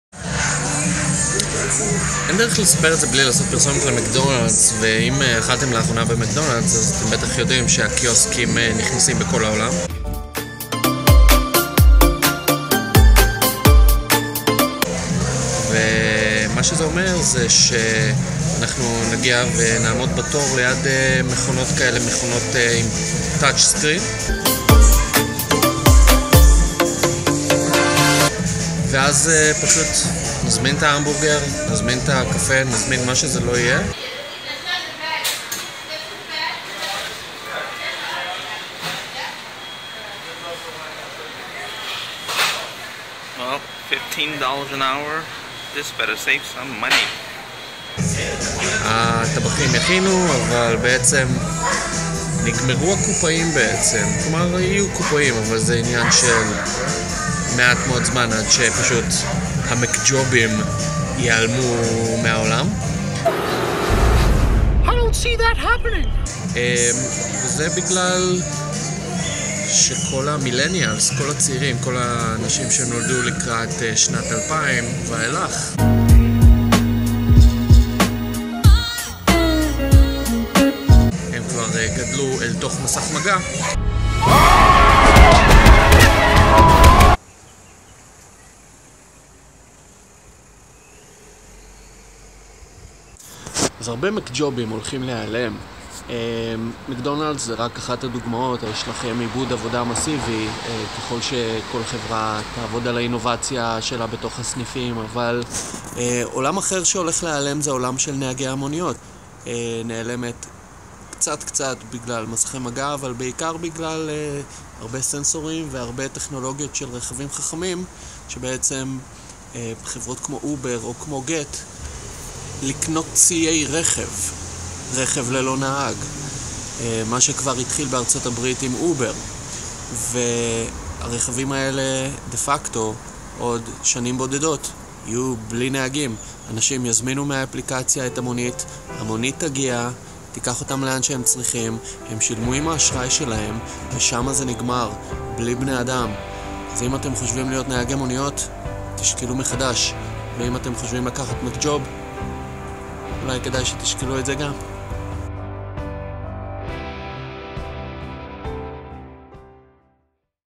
הקשר בין מקדונלדס גט טקסי ואובר- עתיד העבודה והמקג׳ובים
בדרך חזור מהנסיעה שלי צפונה עצרתי במקדונלדס, זה העלה לי כמה מחשבות שצילמתי בוידאו, על החיבור בין טכנולוגיה למקג׳ובים. מפה לשם, זה מתחבר גם לחברות כמו Gett ו- Uber - כמה מילים על עבודות שהולכות להיעלם. ולוג מס׳ 2.
ובעניין אחר- אני מתלבט לאן להעלות את הסרטונים- אם לכאן או לפייסבוק- וכרגע אני מעלה לשתי הפלטפורמות. איפה אתם מעדיפים לראות וידאו? אשמח לפידבק.